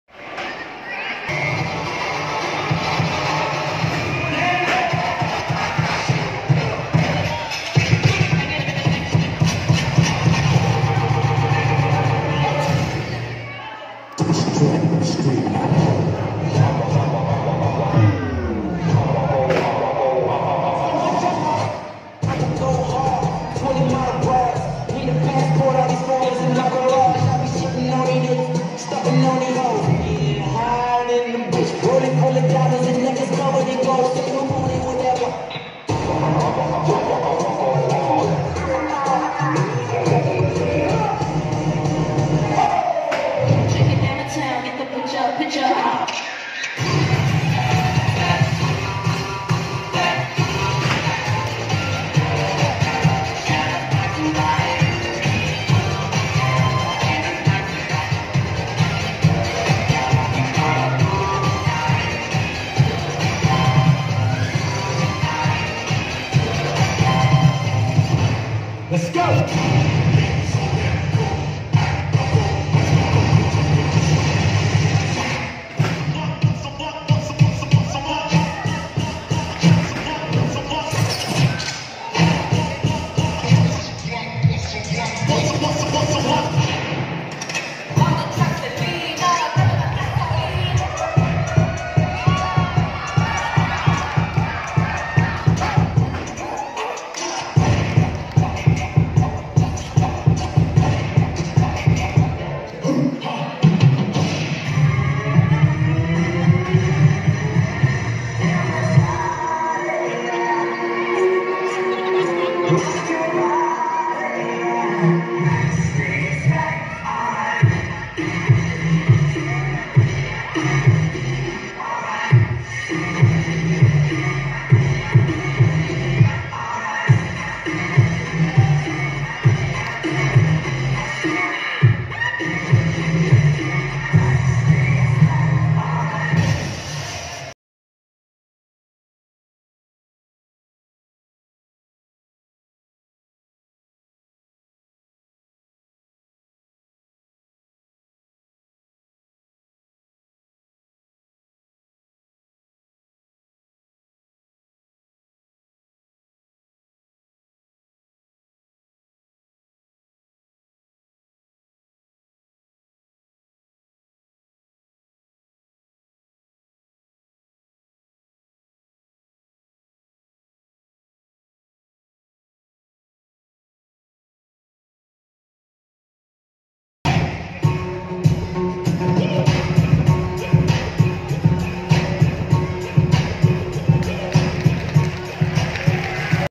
Free Movers[Tay-Ac] Kannawidan dance craze competition season 6[school]elimination round Jan.18,2020
Kannawidan dance craze competition is now on it's 6th season.
Congrats to all. Specially sa mga nakapasok para sa finals.

P.S.
Kung mapapansin nyo is nawawala yung music. Ibig sabihin po is copyright music po sya. Kaya pinapipili po ako ni YouTube kung mawawala yung boung video/s o mute lng yung my part na copyright music, Kaya pinili ko yung mute kesa mawala yung boung video/s.
Note: Hindi po boung video/s yung walang music kundi yung my part lng na copyright music ang mamumute. Salamat sa pag'unawa.